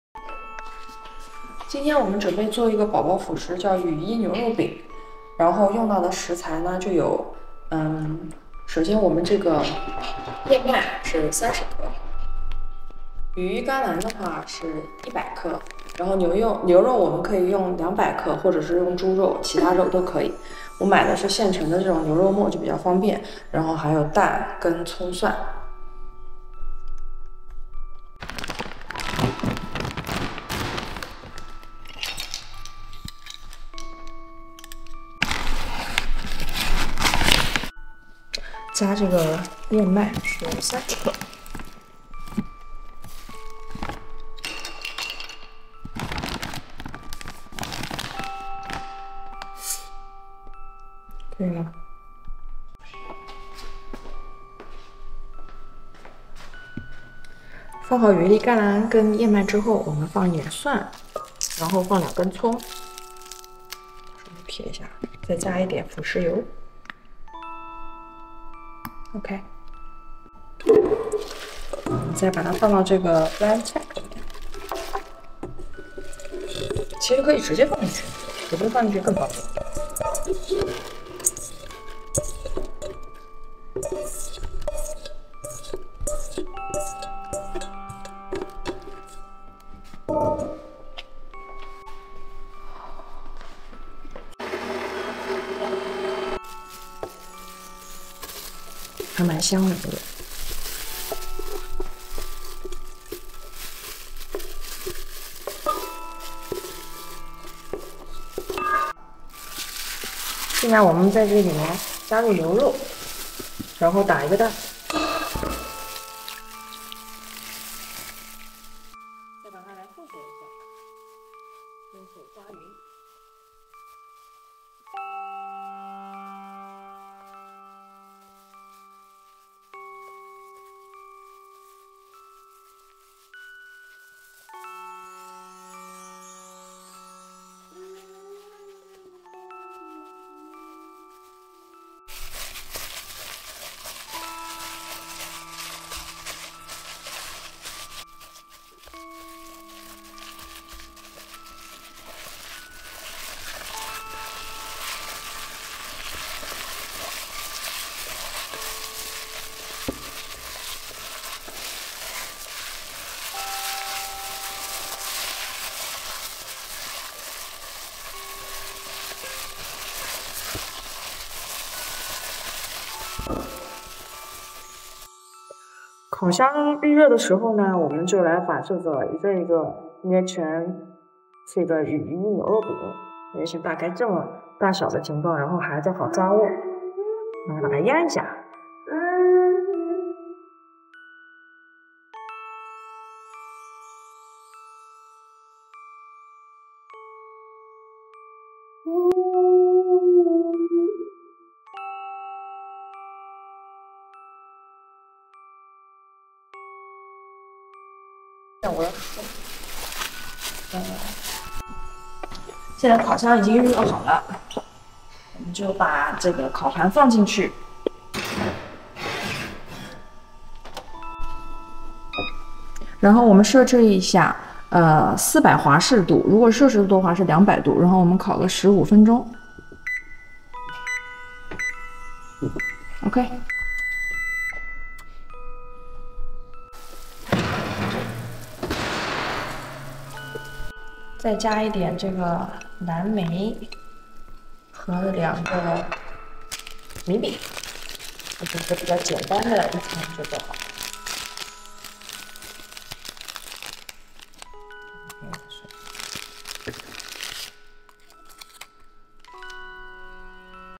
輔食副食品羽衣牛肉餅
婴儿羽衣牛肉饼适合所有宝妈喝奶爸们制作哦，
配料;牛肉沫200g、羽衣甘蓝100g、葱一把、蒜头5个、橄榄油